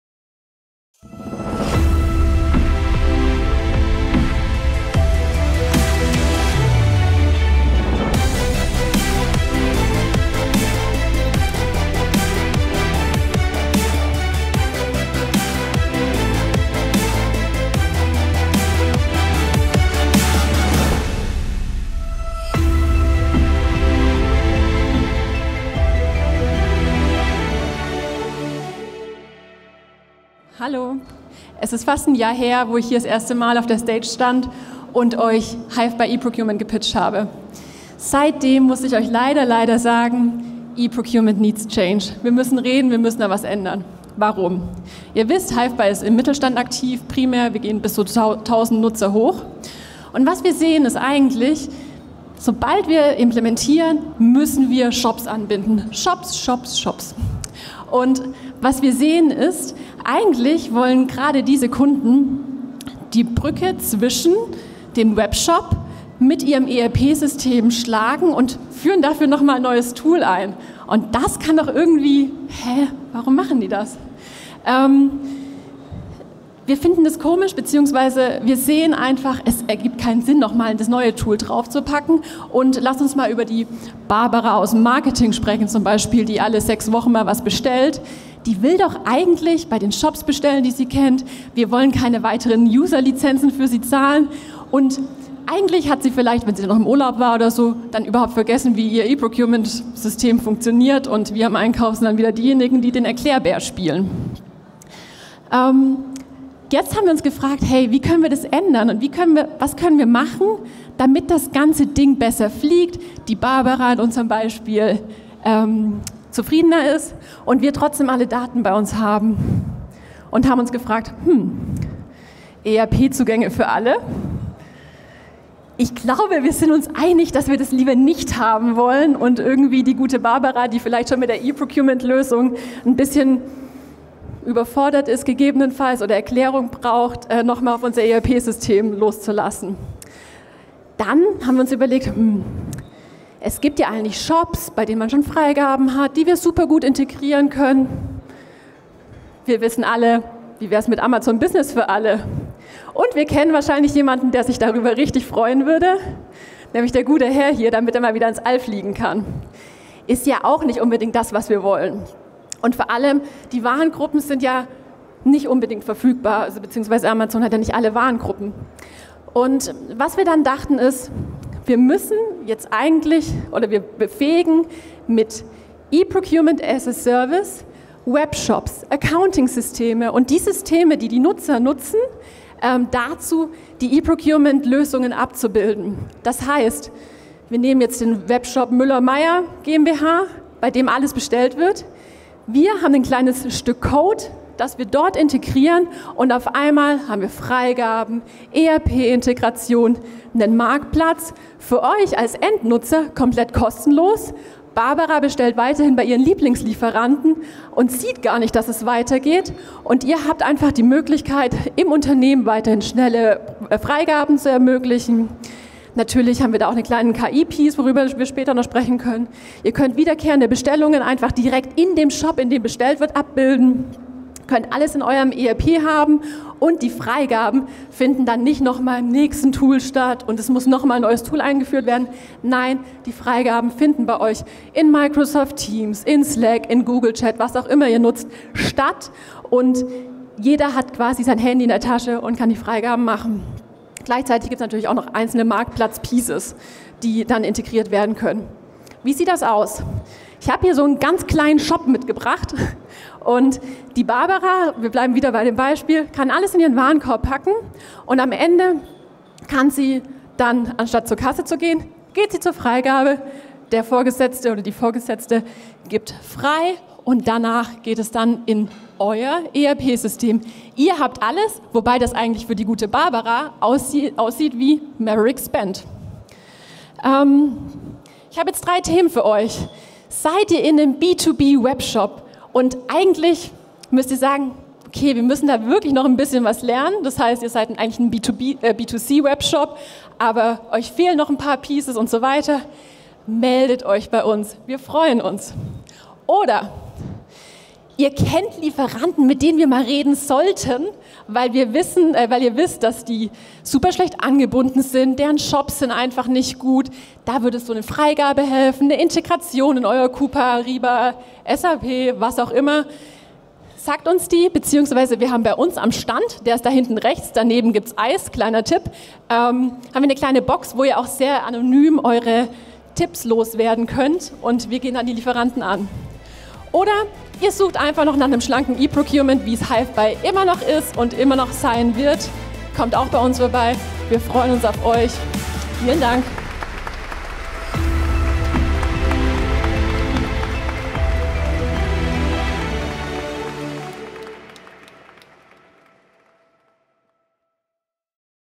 Procurement Summit Startup Awards - Hivebuy
SPEAKER
Bettina Fischer (Hivebuy)

MODERATION
Sebastian E. Merget (Procurement Summit)